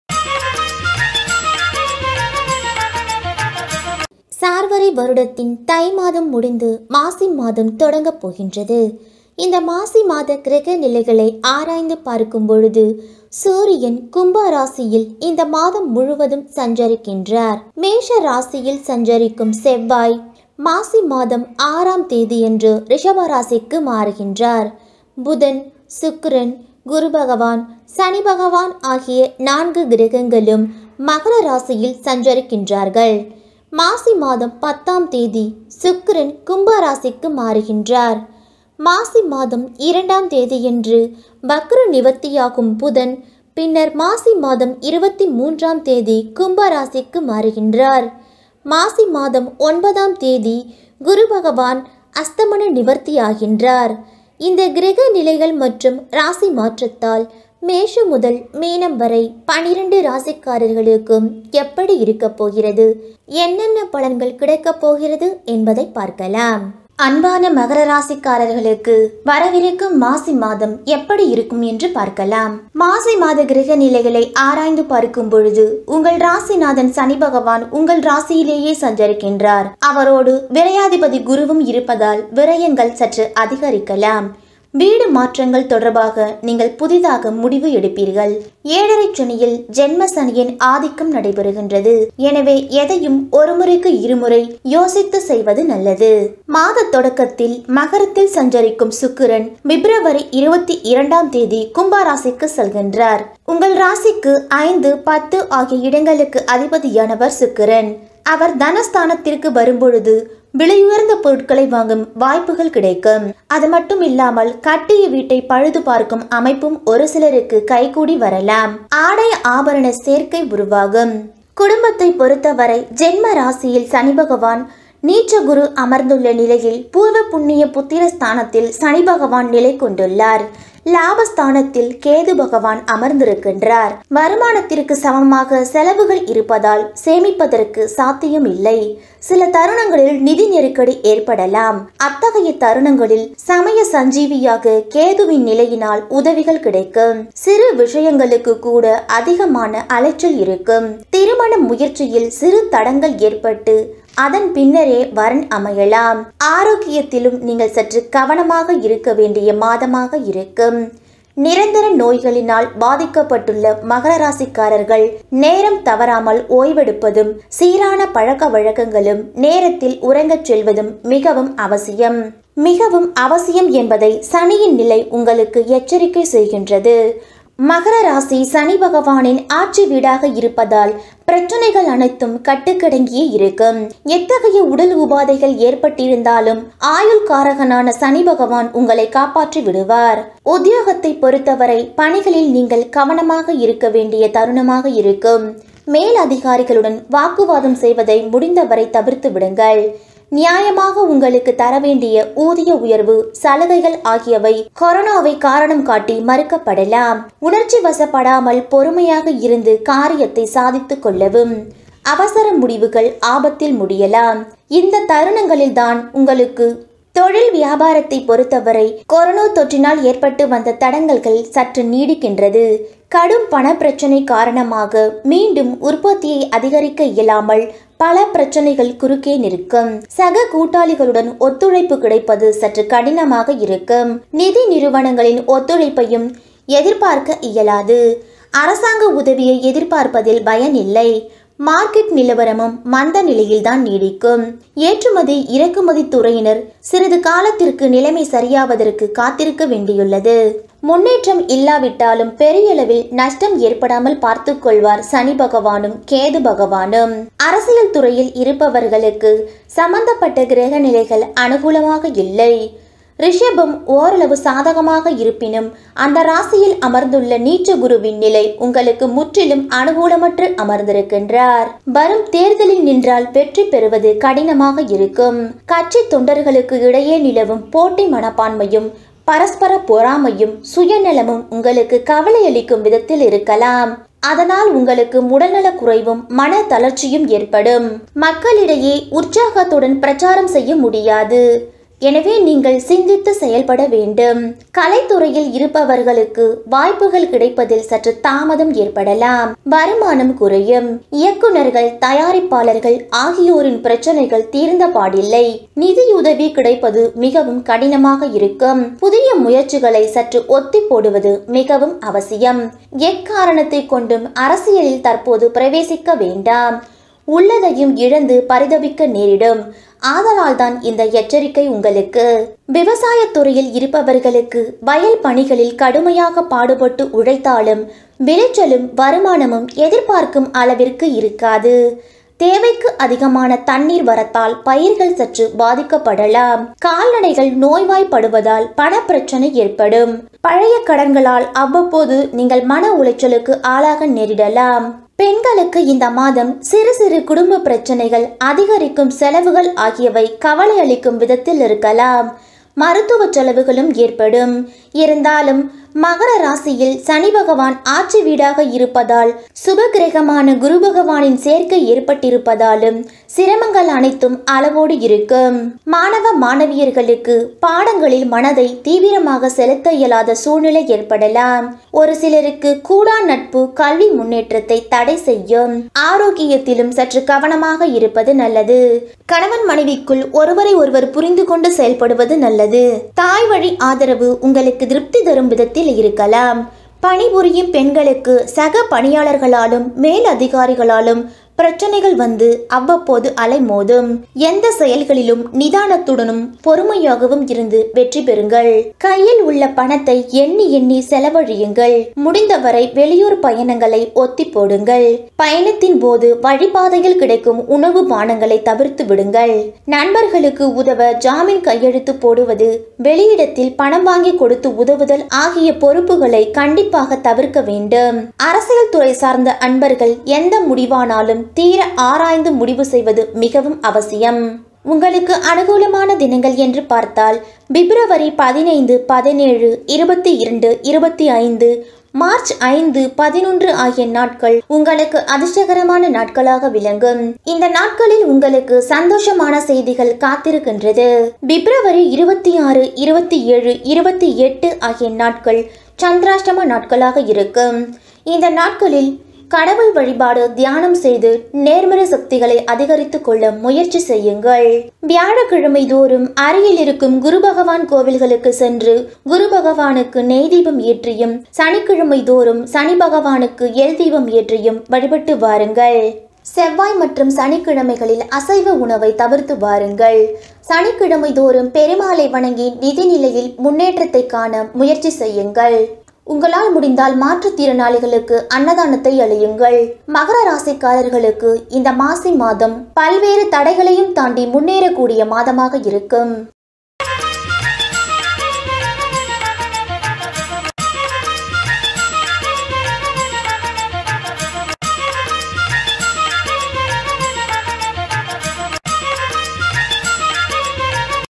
Maasi matha rasi palan 2021 | Makaram (Capricorn) | மகரம் | மாசி | February month prediction
astrosivan Maasi# matha rasi palan#| Makaram#